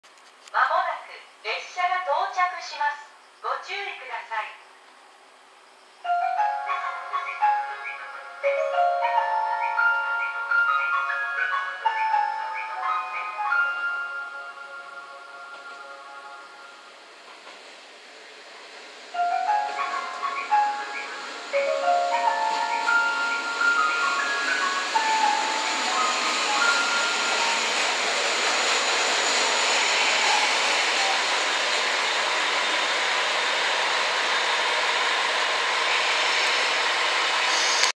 列車到着メロディー 芸備線 上り普通列車 国鉄キハ40系気動車（2両編成） キハ47形96 + 47形2022 広島 出発 JR西日本 下関総合車両所 広クチ 00758
芸備線 上り普通列車 国鉄キハ40系気動車（2両編成） キハ47形96 + 47形2022 広島  出発 JR西日本 下関総合車両所 広クチ 00758